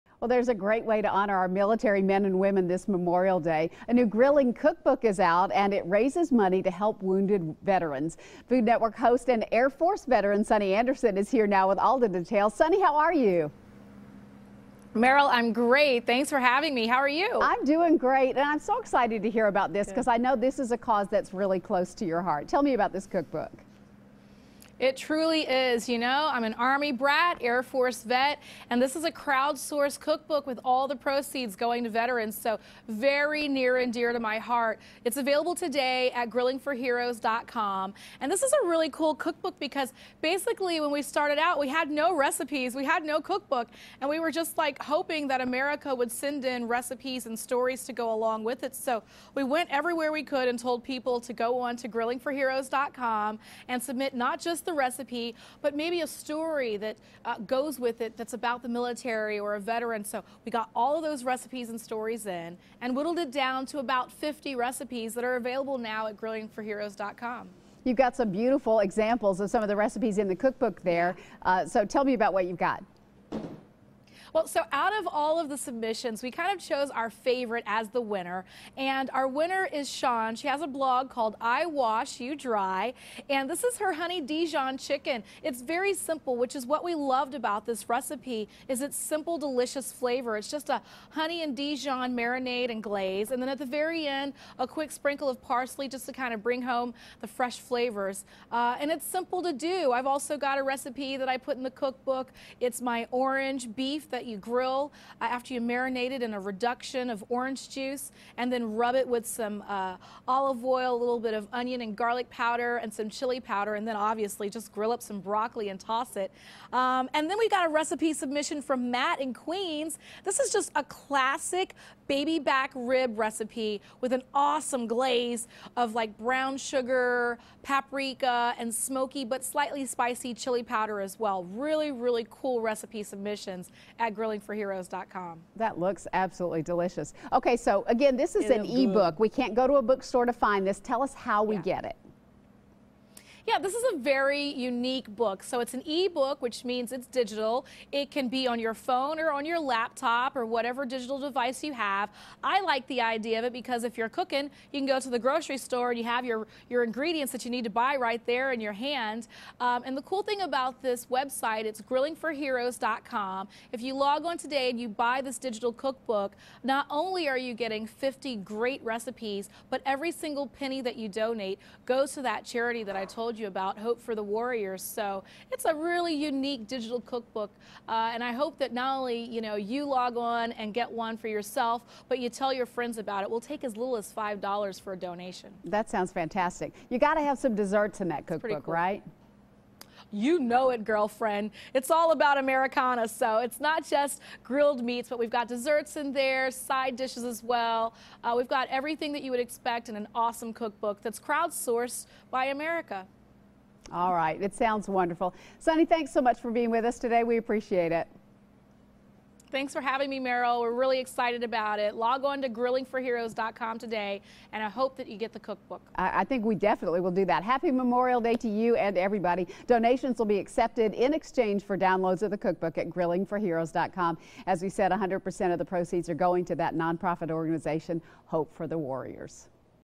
New Cookbook by Food Network Star Sunny Anderson
Grilling for Heroes ideas for great summer recipes